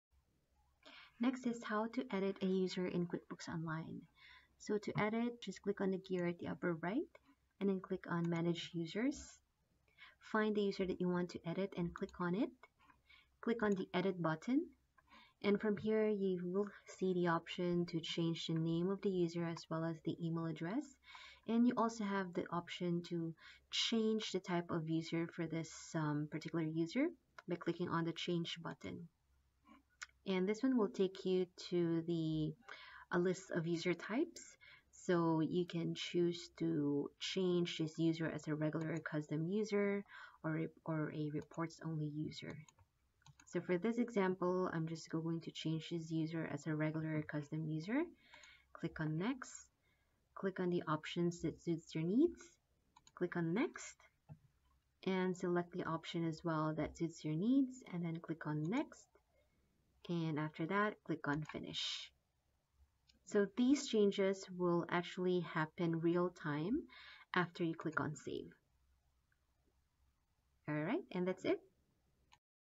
QuickBooks Tutorial: How to edit a user
Add a QuickBooks Online user

Step 1: Invite your new user
Select Settings ⚙, then Manage users. If you can’t select this, you don’t have permission to manage other users. Contact an admin user for help.
Select Add user.
Select the user type you want to create.  More options will appear on the screen depending on the user type you select.

Seeing a user limit?

If you see a user limit message, it’s because QuickBooks Online has a maximum number of users depending on your subscription. Learn about limits and what you can do to add a user.

Step 2: Have your user accept the invite
Your new user will quickly get an email from . It has your business’s name and a message that includes “Ready to get started? Let's go!"

Have your new user select the "Let’s go!" link in the email, which takes them to a sign in or account creation page. They need to sign in or enter info to create a new account.

This could be for a different product. If they’re not sure how, they can select I forgot my user ID or password at the bottom of the screen.

Manage user role or permissions
Sign in to QuickBooks with a user profile that has permission to manage users.
Select Settings ⚙.
Select Manage users.
Find the user you want to edit. Then select Edit in the Action column.
In the User type drop-down ▼ menu, select the new user type.
Select the user settings, if applicable.
Select Save.
Ask the user to sign out and sign in again into QuickBooks Online to see the updates.
Certain users don't count towards your user limit, like the Reports only or Time tracker roles. You can't edit these user permissions. If you need to edit that user, simply delete them. Then add them again with the correct role.

Delete a user
Keep in mind, deleting a user is permanent. But you can still view their history in the audit log.

Select Settings ⚙.
Select Manage users.
Find the user you want to delete. Then select the arrow icon in the Action column.
Select Delete.
In the confirmation window, select Delete again.